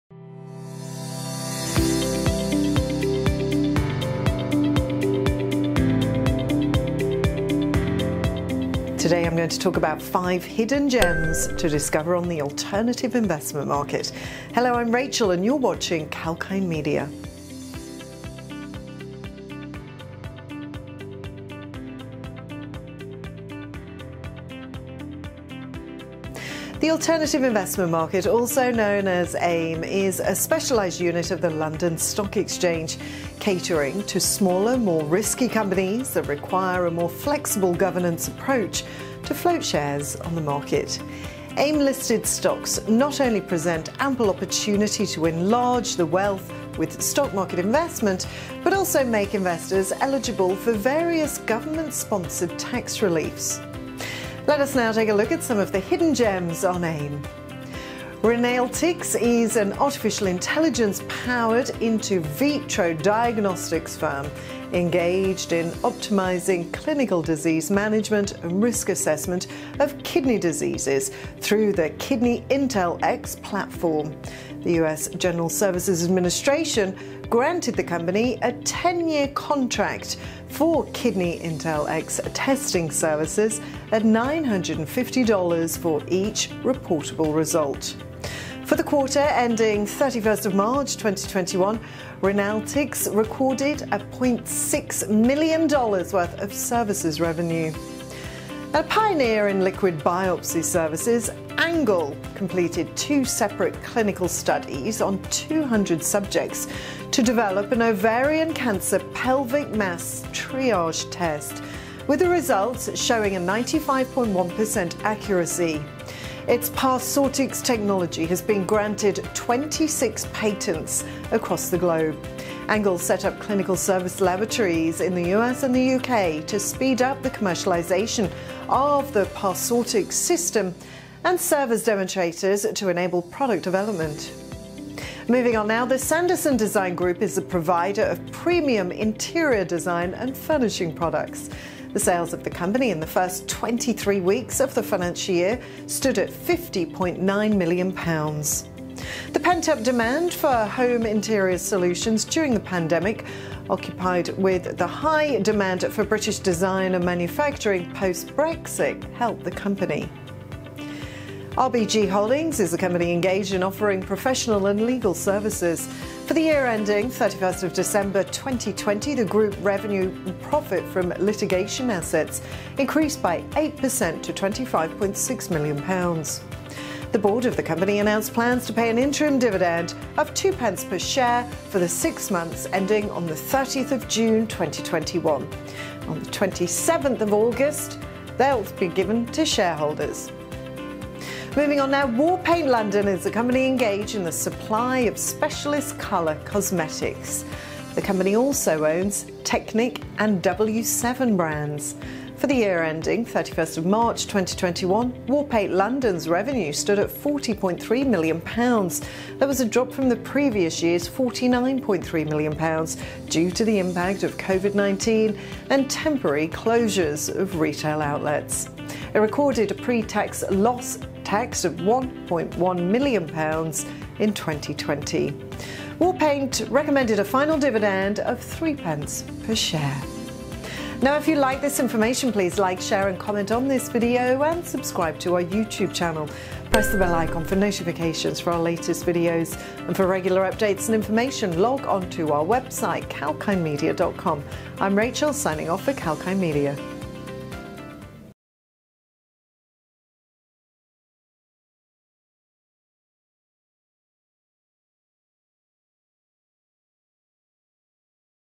Top 5 AIM Stocks with 100% YTD returns
AIM listed stocks not only present ample opportunity to enlarge the wealth with stock market investment but also make investors eligible for various government-sponsored tax reliefs. There are many AIM listed stocks that have already delivered more than 100 per cent of return in the year so far. Watch this video to find out more about these AIM listed stocks.